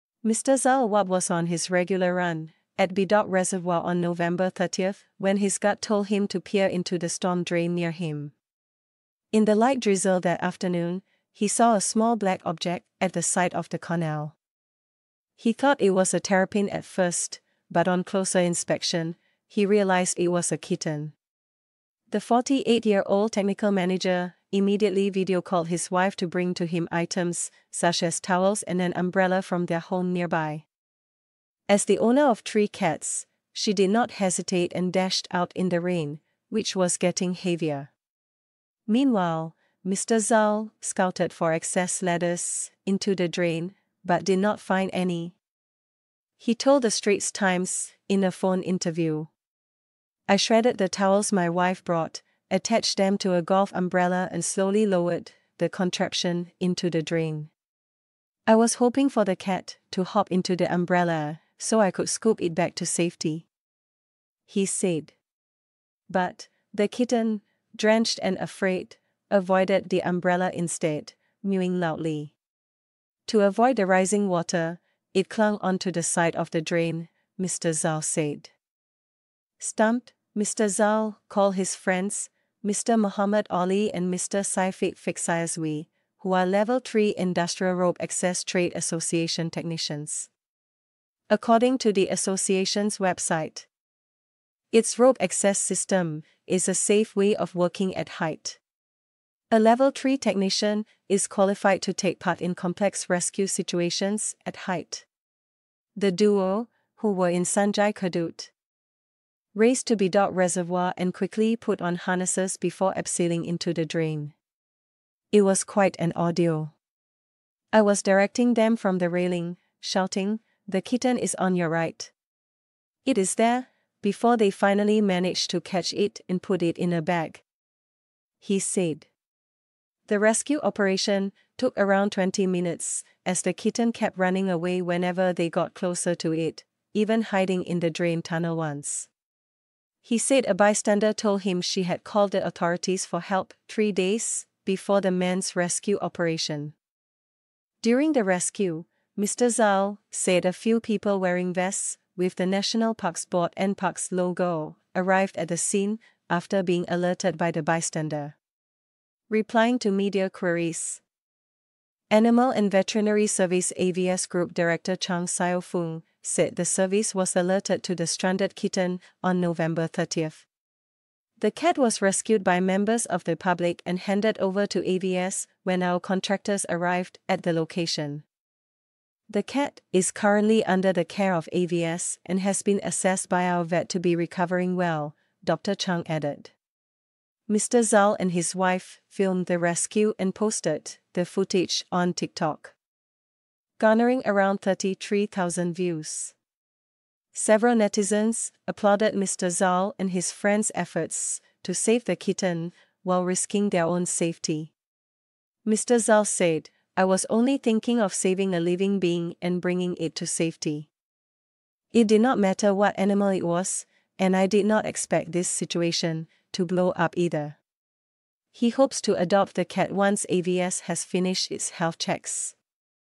Friends rescue kitten in Bedok Reservoir storm drain during heavy rain
💗📌 Singapore